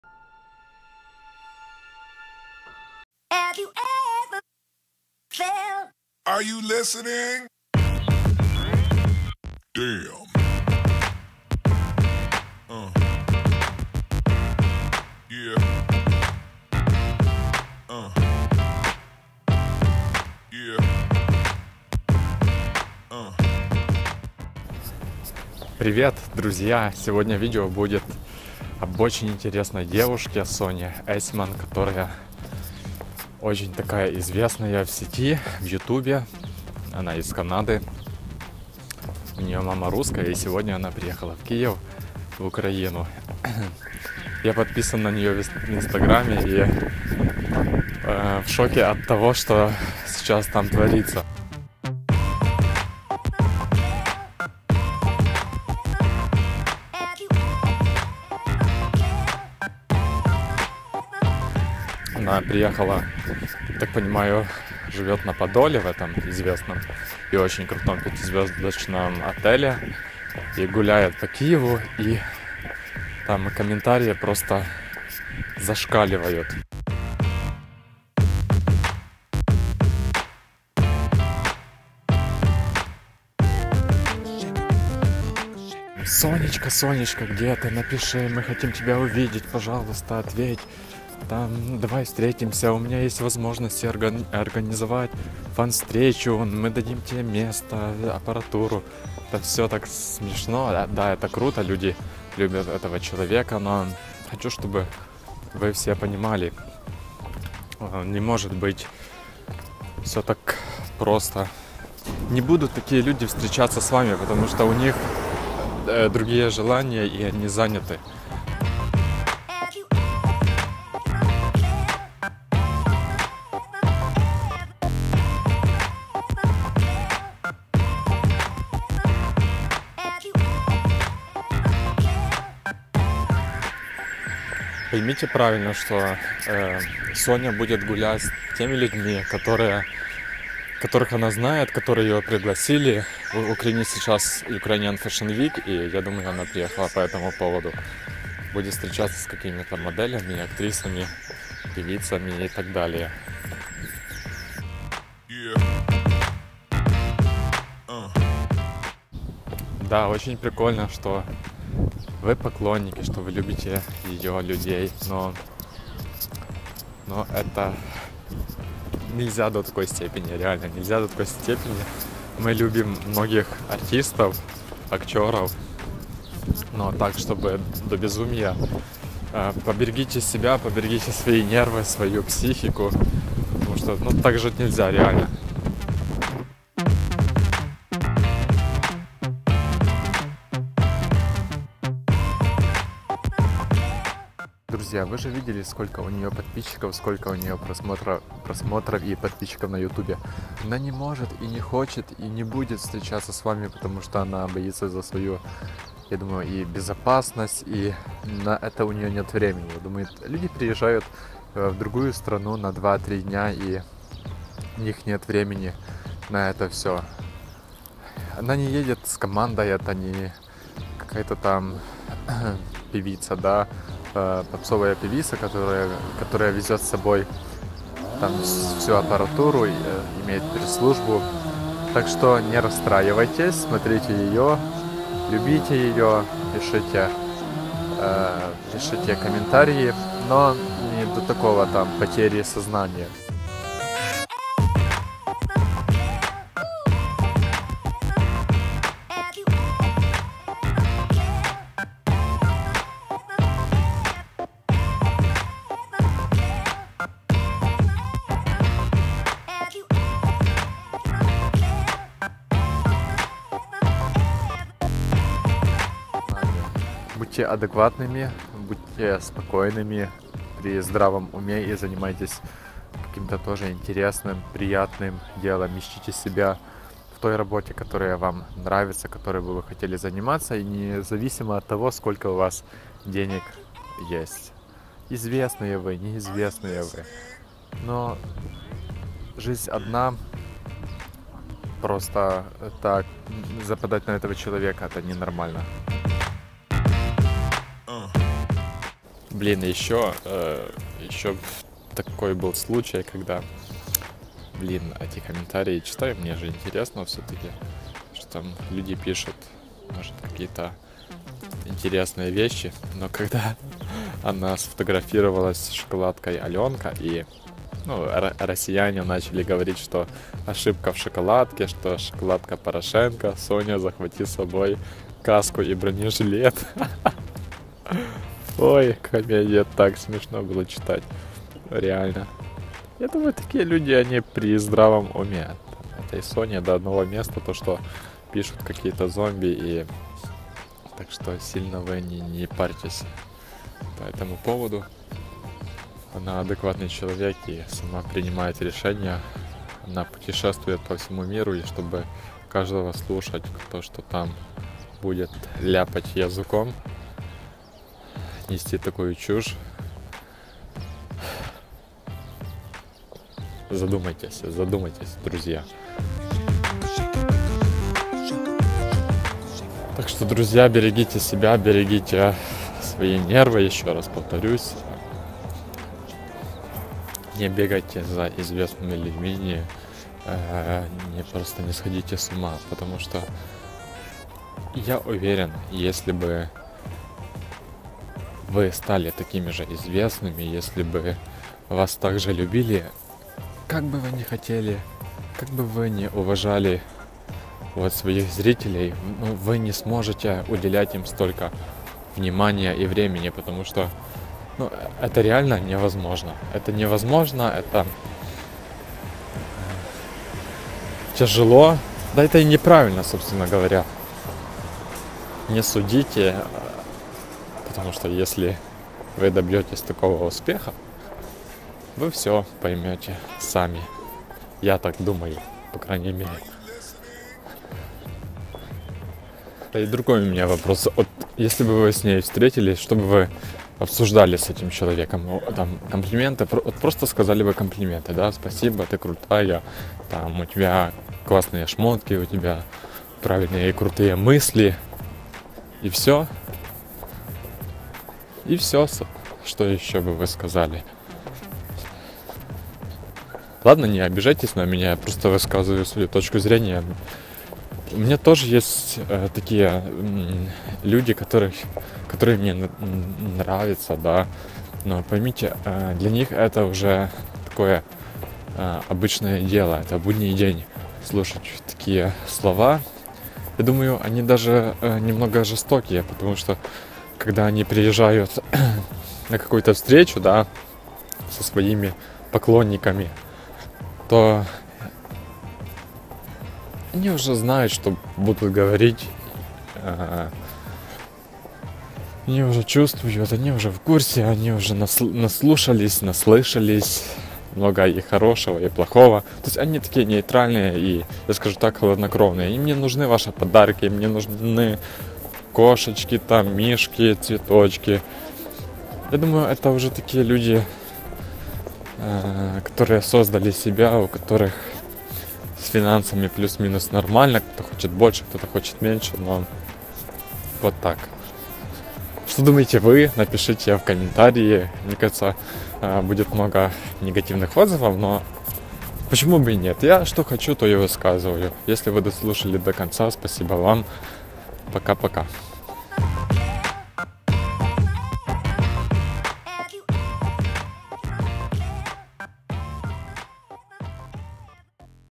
Соня Есьман приехала в Киев,we love you, but...............
Все ищут известную Соню Есьман в Киеве. Думаю она приехала на UFW, но не всегда наши желания совпадают с реальностью, так что  желаю нам всем терпения и понимания!!! Успешные люди не могут быть доступными всем нам, таков закон жизни... p. s. Фанаты Сони,  не злитесь сильно на меня, она мне тоже нравится!!! Соня Есьман в Киеве
Жизнь в Киеве/Life in Kiev
UA Дор
Соня Есьман в Киеве, Esman Sonia